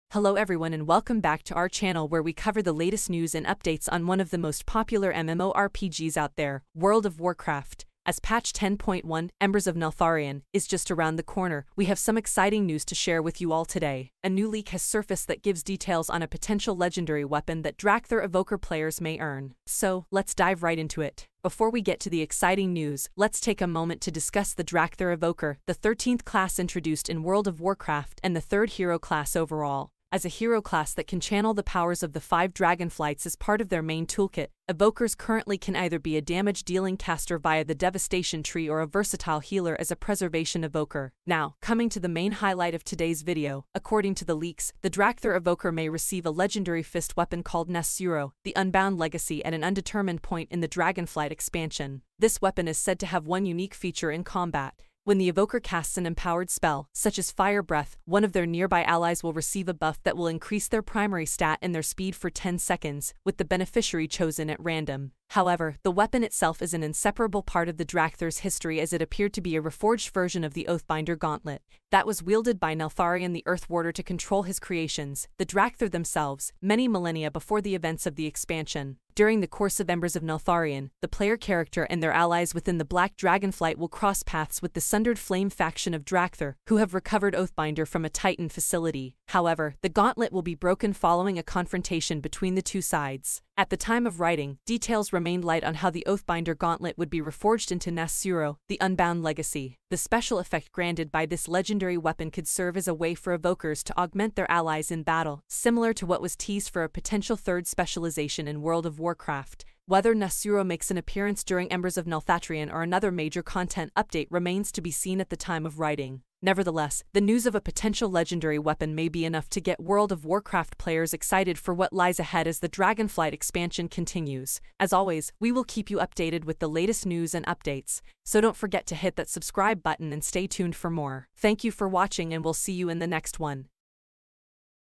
World of Warcraft Leak May Point to Legendary Weapon for Evokers
In this video, we'll be diving into the latest leak in the World of Warcraft community that could potentially reveal a new legendary weapon for Dracthyr Evokers. With the release of Patch 10.1, Embers of Neltharion, just around the corner, this leak has the WoW community buzzing with excitement. We'll take a closer look at the details of the leak, including what the weapon could look like and how it may benefit Evoker players. We'll also explore the history of the Dracthyr Evoker and their ties to the Dragon Isles, as well as the potential implications of this leak for future updates in the Dragonflight expansion. Whether you're a long-time WoW player or just a curious gamer, this video is sure to give you some exciting insights into the world of Azeroth.